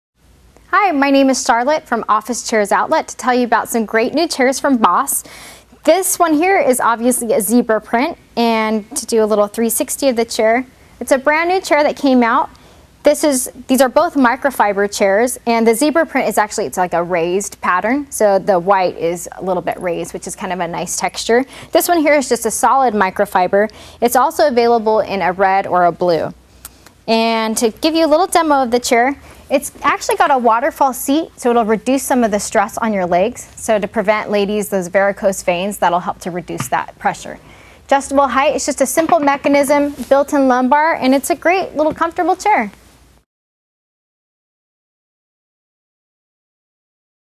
Boss Zebra and Pink Microfiber Chairs
Boss Microfiber Office Chairs. Available in many colors and prints. Zebra and Pink shown here.